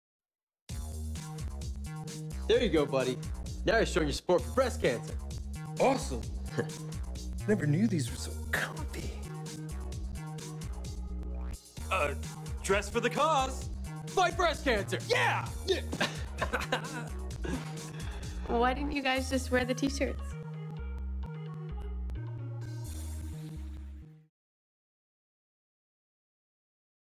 Dress For The Cause - The Breast Cancer Society of Canada
Wear your SUPPORT BRA and raise money for a great cause! Wednesday, October 16th, 2013 join businesses all over Canada to SUPPORT THE FIGHT AGAINST BREAST CANCER!